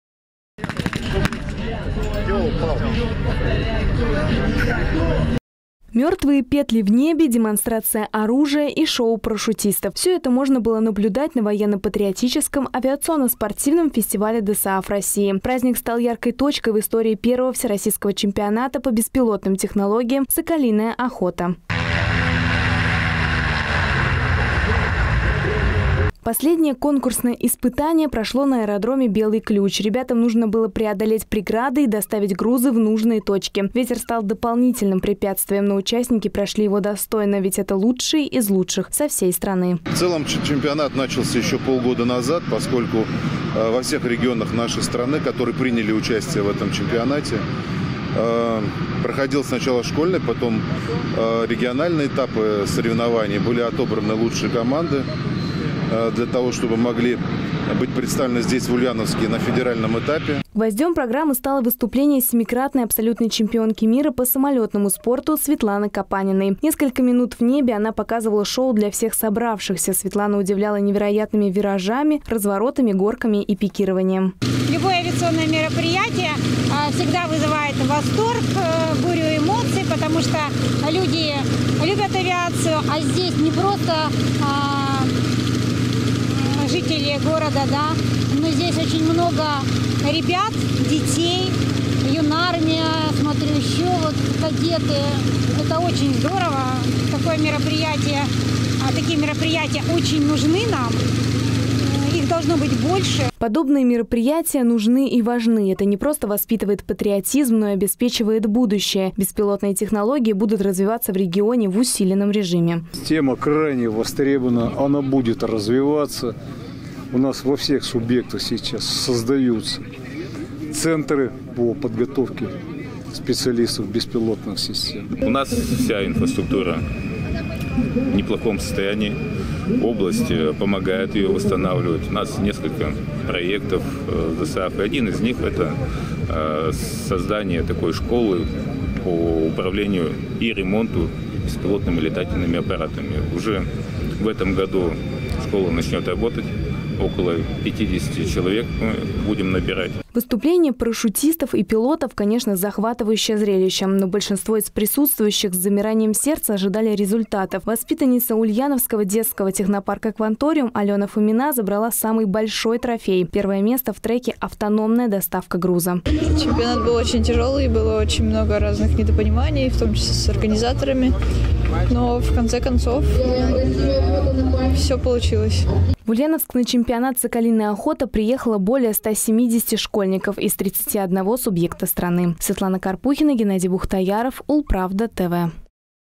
Ульяновцы забрали трофеи на «Соколиной охоте»
В регионе завершились первые Всероссийский чемпионат по беспилотным технологиям «Соколиная охота». Торжественный финал прошел на аэродроме «Белый ключ». По итогам всех испытаний определены победители по каждому треку в разных возрастных категориях.